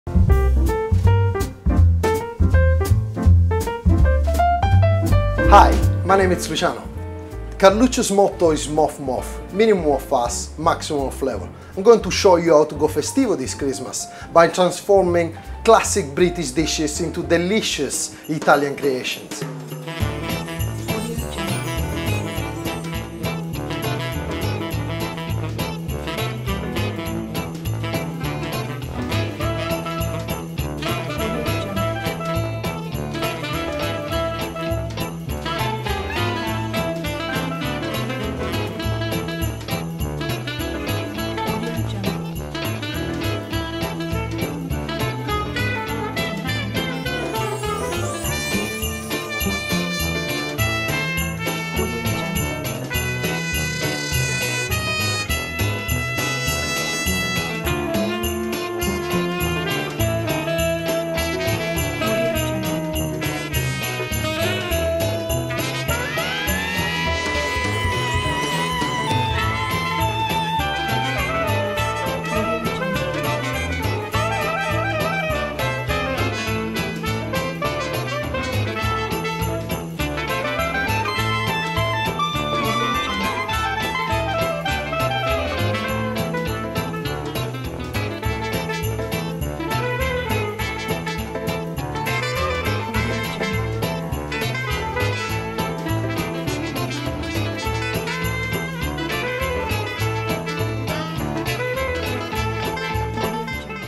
Italian Festivo Christmas Recipes
Bored of the same old Christmas dishes? Tired of turkey? Give them an Italian twist with our very own Chef’s take on Christmas. GoFestivo Wow your guests and download our recipes